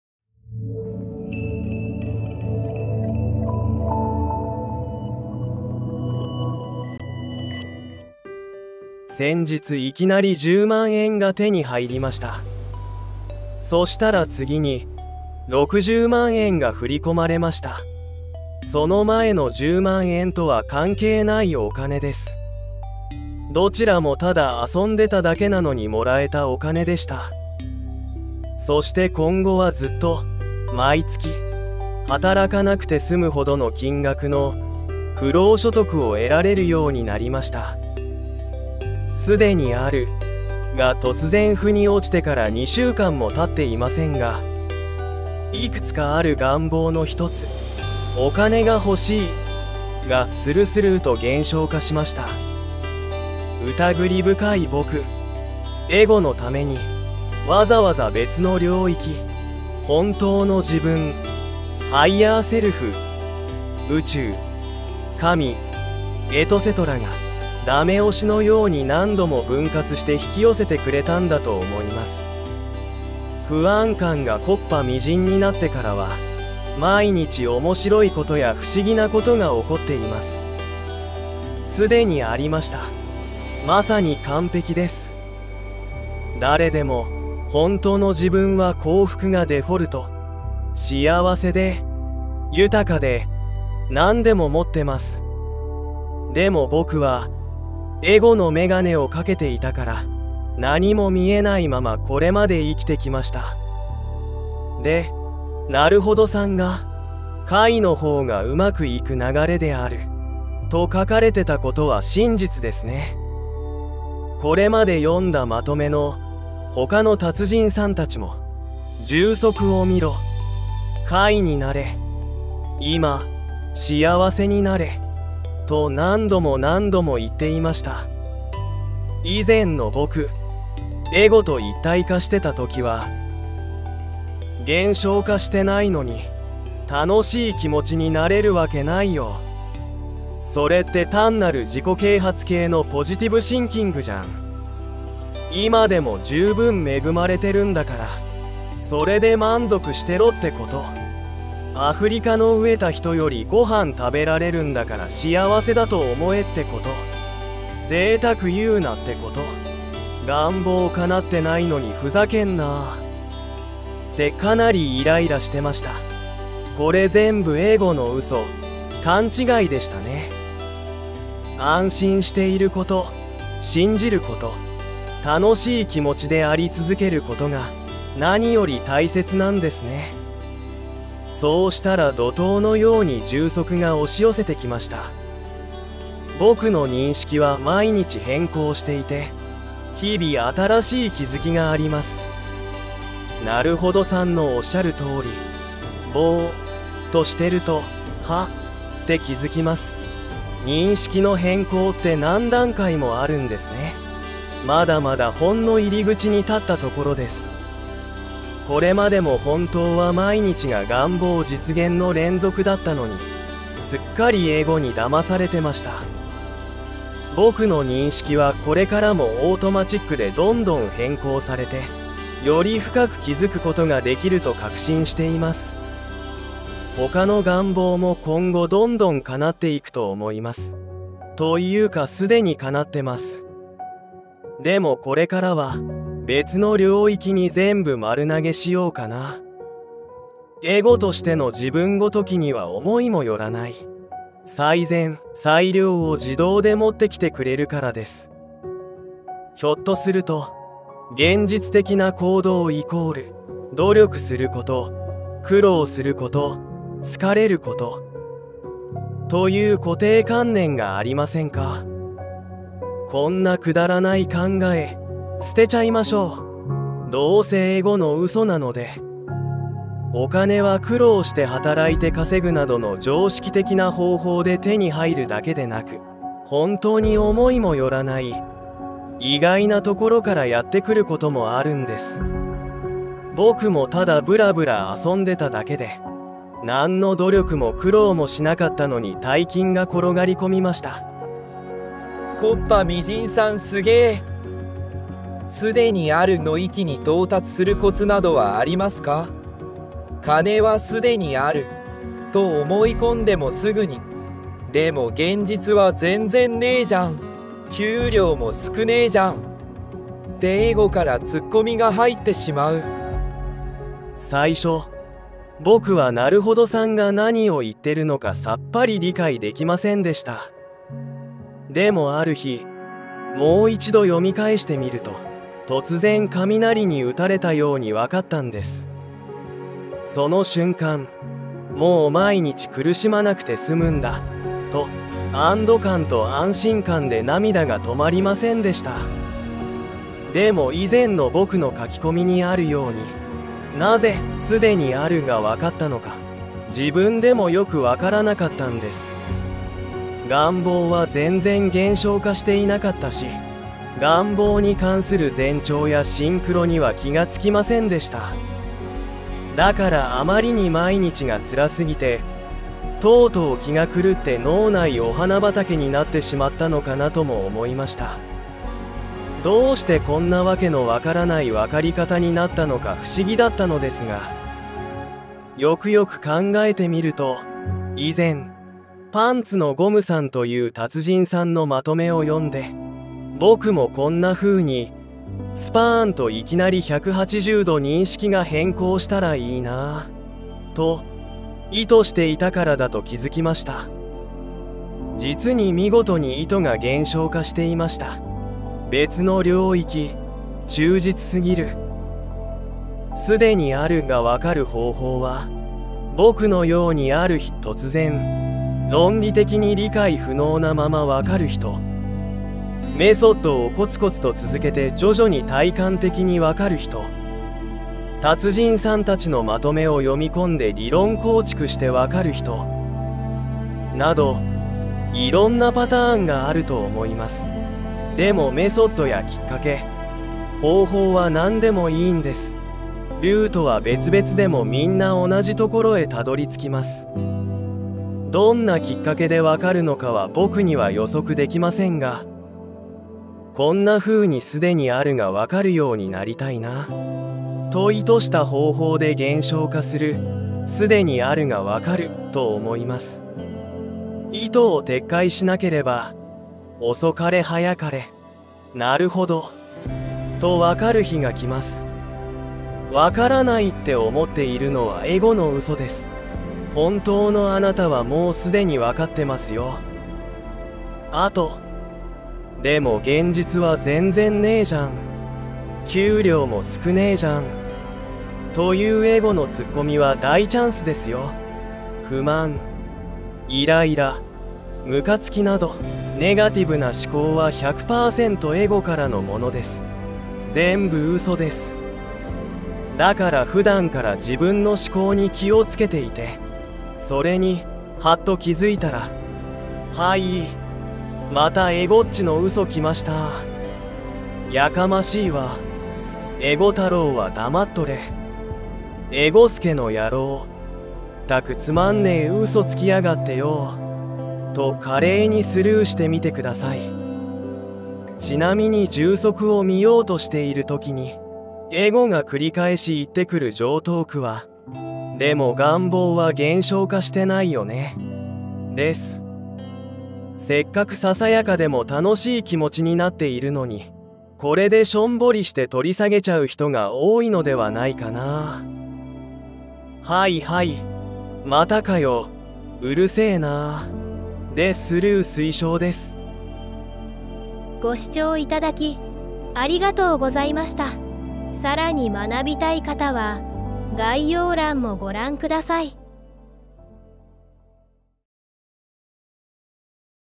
【こっぱみじんさん②】認識変更してから2週間足らずで、大金＆不労所得を得られるようになった
さらに学びたい人は
潜在意識ちゃんねるオンラインサロン

「潜在意識」「引き寄せの法則」の、真理を掴んだ達人さんたちのお話です。

●こっぱみじんさんが認識変更したキッカケとなった「なるほどさん」の再生リストです。

「既にある」がわかってから2週間足らずで大金＆不労所得を得られるようになった、こっぱみじんさんの動画です。
認識が毎日変更していて、日々新しい気付きの中のお言葉です。

※潜在意識ちゃんねる様よりコンテンツ使用許諾取得済み
※音声読み上げソフト使用
※素材使用　Pixabay様
　（↑上記以外は動画制作ソフトから提供される素材を使用しています）